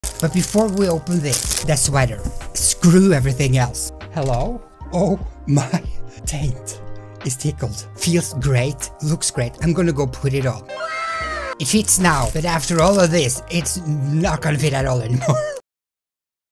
My neighbor Totoro sweater! Studio Ghibli
I got a Studio Ghibli my neighbor Totoro sweater!
here is a quick review and show. Full video with the sweater and tasting all the Japanese snacks from the mystery bag In the attached video!